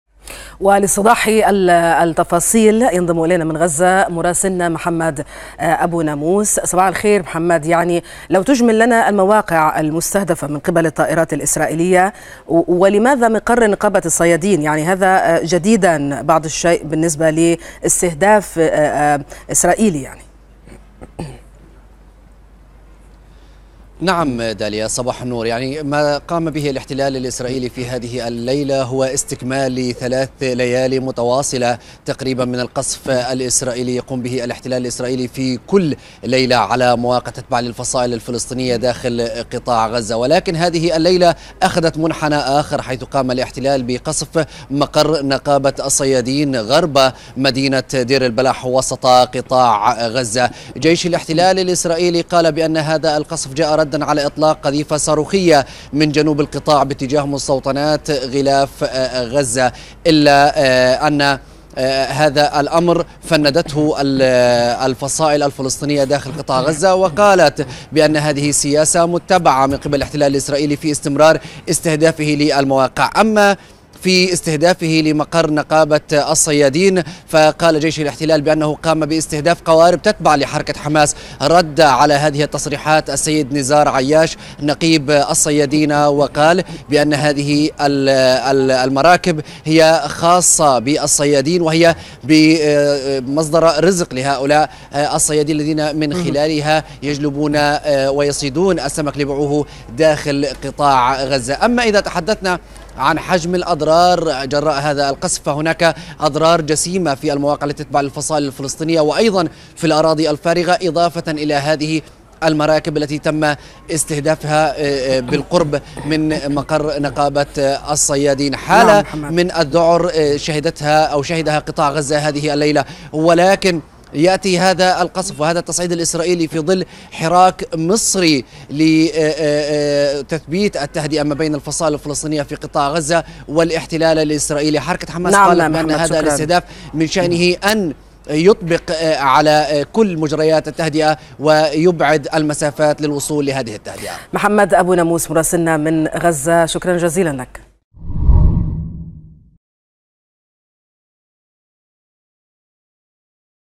مراسل الغد: استهداف نقابة الصيادين ضمن غارات الاحتلال على قطاع غزة
استشهد شاب فلسطيني برصاص قوات الاحتلال الإسرائيلي، فجر اليوم، على حاجز عسكري في منطقة غور الأردن في أريحا وسط الضفة الغربية
المزيد من التفاصيل يتحدث مراسل قناة الغد من غزة، محمد أبو ناموس، مع الإعلامية داليا نجاتي، عبر النشرة الإخبارية.